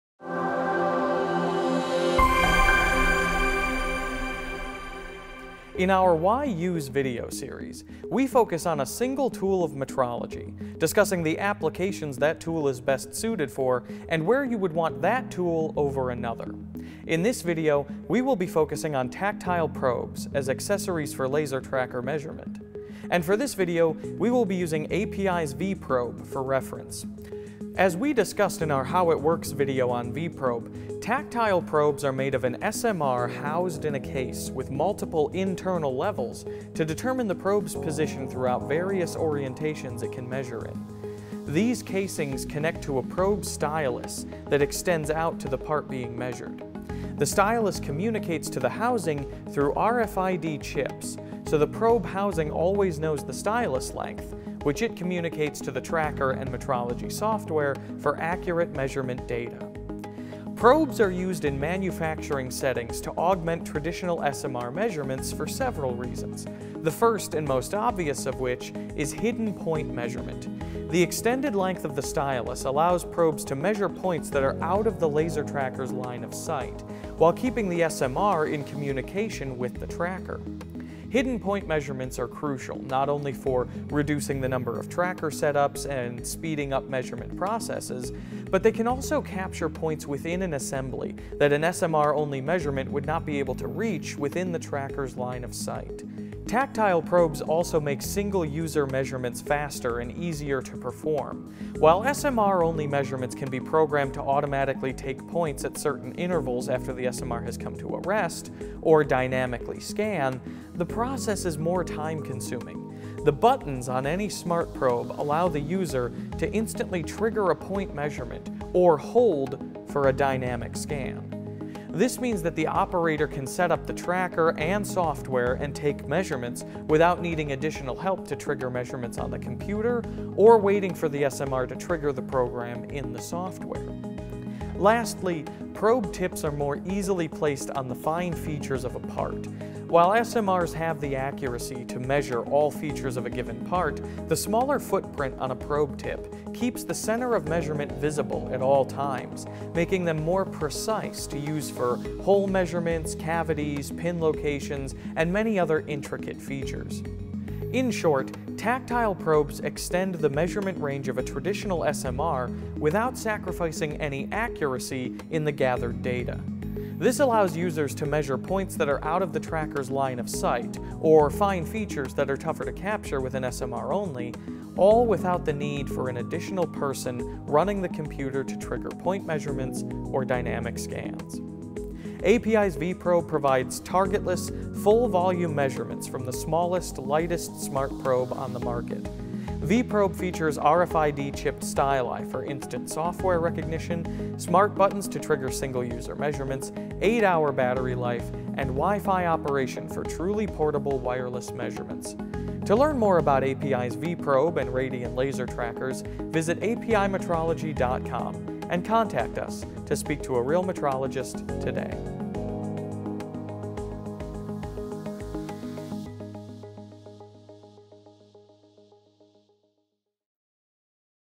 Why Use? | Tactile Probes
In our Why Use? video series, we focus on a single tool of metrology, discussing the applications that tool is best suited for, and where you would want that tool over another. In this video, we will be focusing on tactile probes as accessories for Laser Tracker measurement.

Tactile probes extend the measurement range of a traditional SMR, allowing users to measure points that are out of the tracker’s line-of-sight or fine features that are tougher to capture with an SMR-only, all without the need for an additional person running the computer to trigger point measurements or dynamic scans.